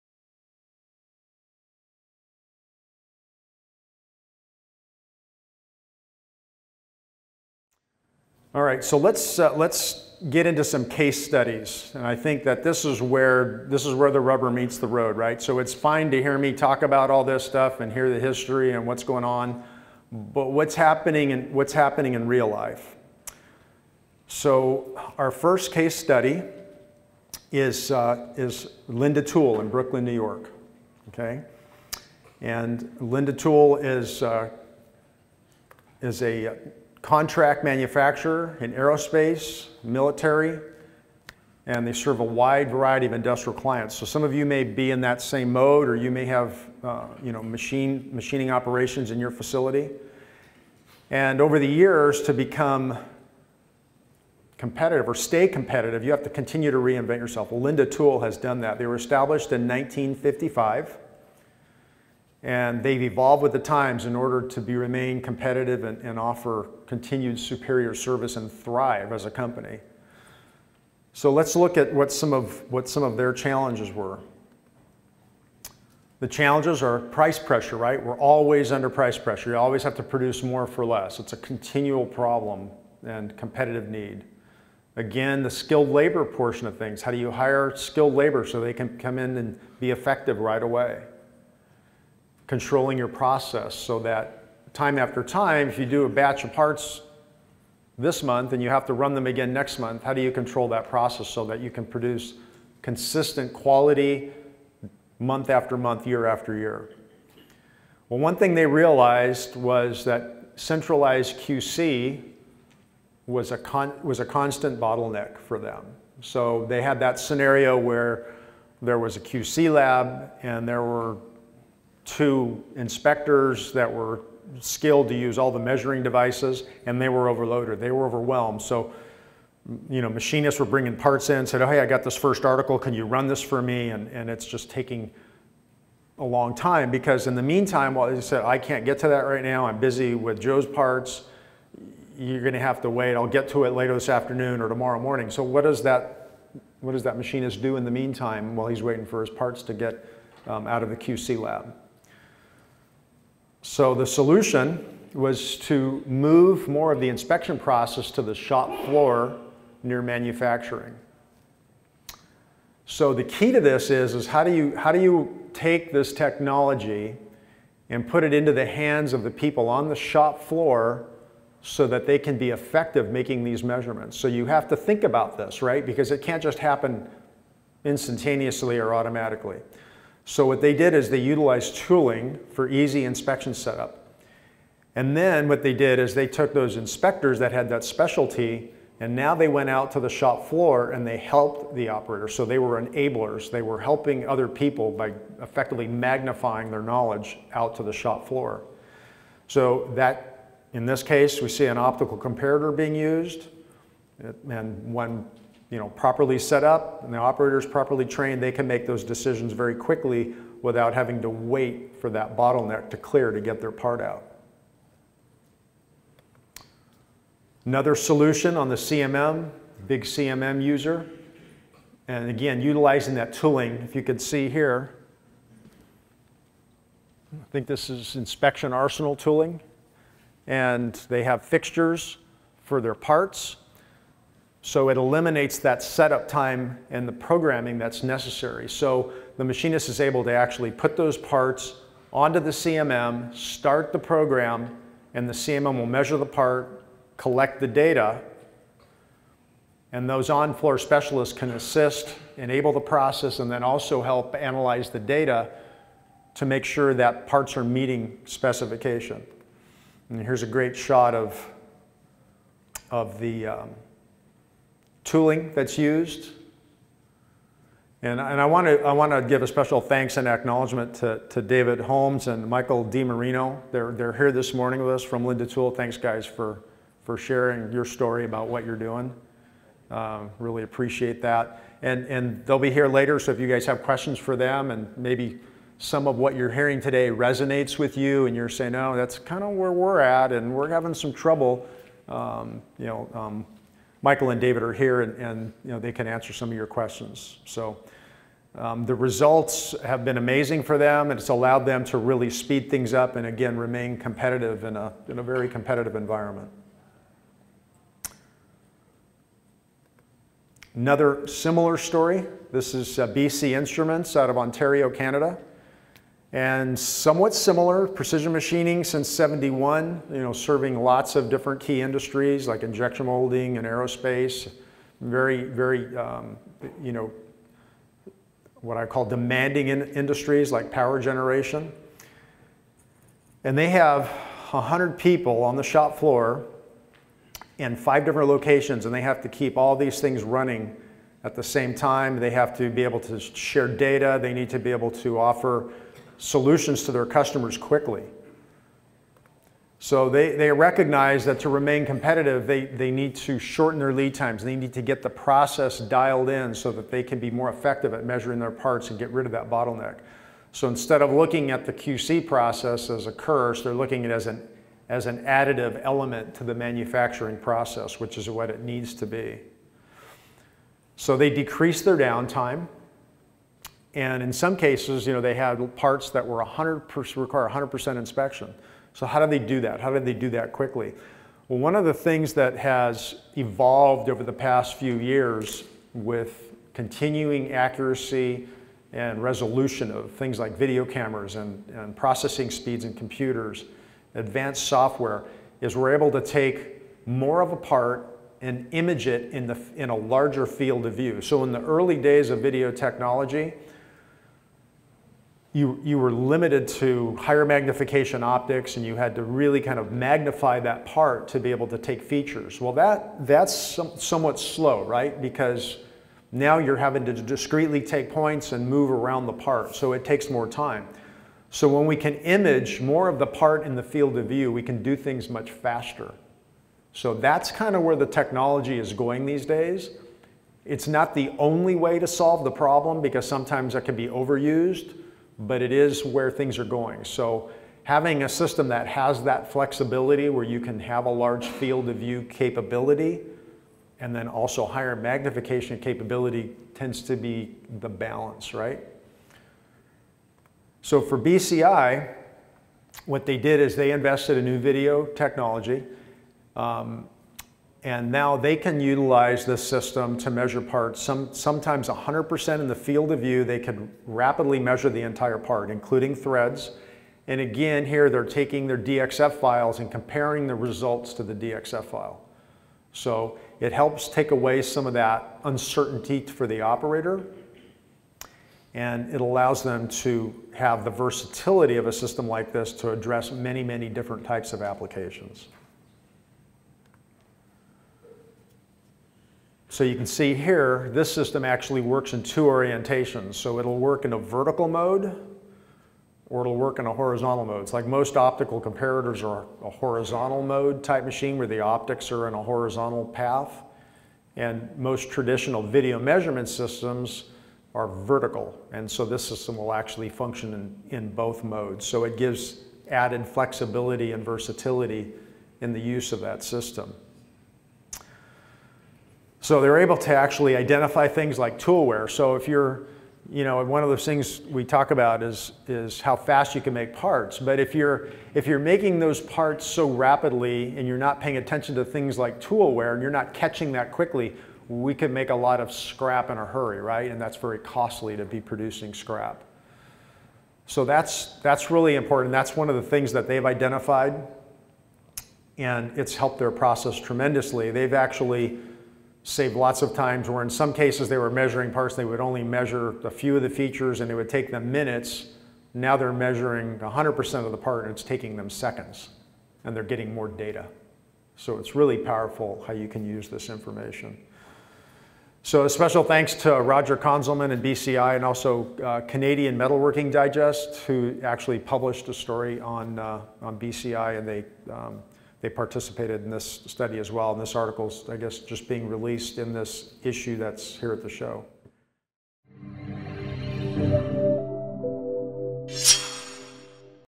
Walk-Up Metrology: Case Studies (Segment 7/10)
Segment 7 out of 10 in our series on Walk-Up Metrology takes a look at several case studies from companies that have introduced Starrett metrology systems into their environment.